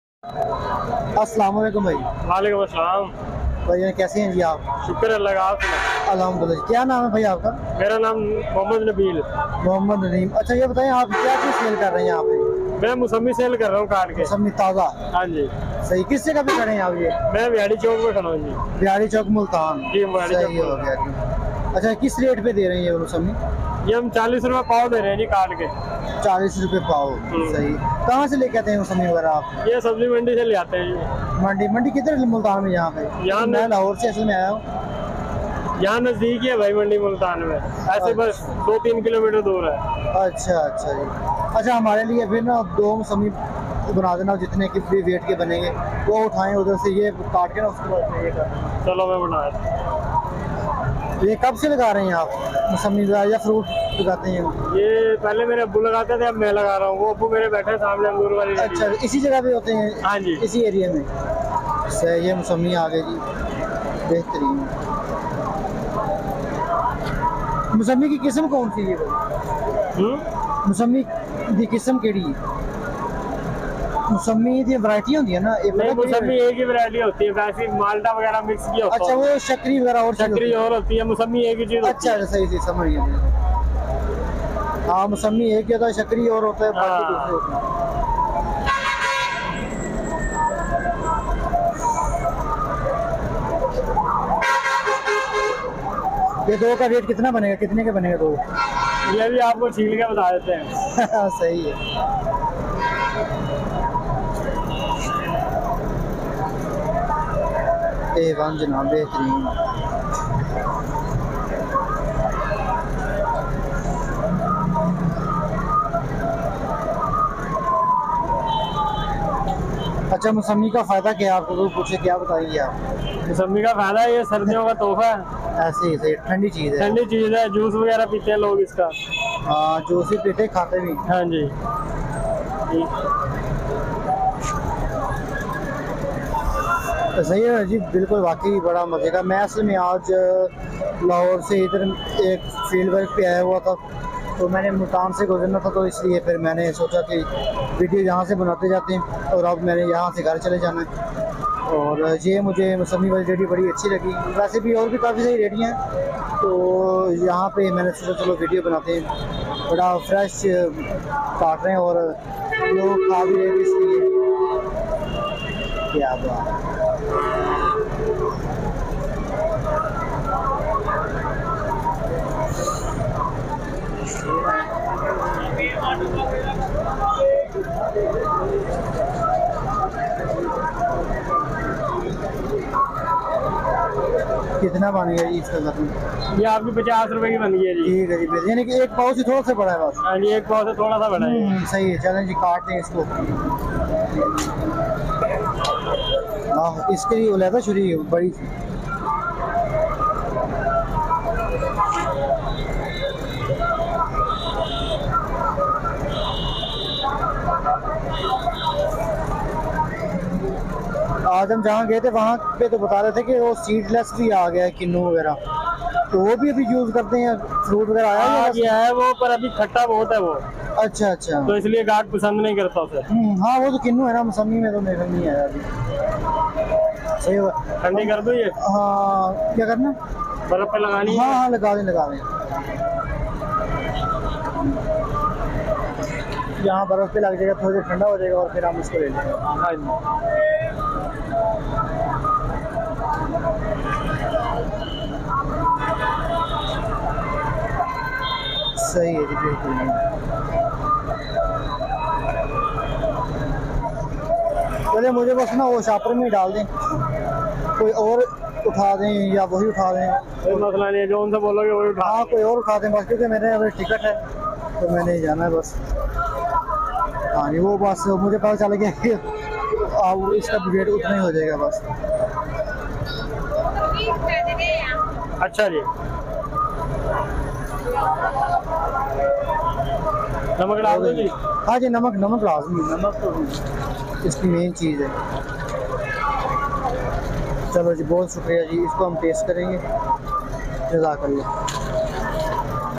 Amazing musami cutting technique in street food Multan | Citrus musami eating
Amazing Musami cutting Technique in Multan city, it is same Everywhere 😂

Price: 40/0.25kg
Taste: 100/100
Location: Vihari Chowk Multan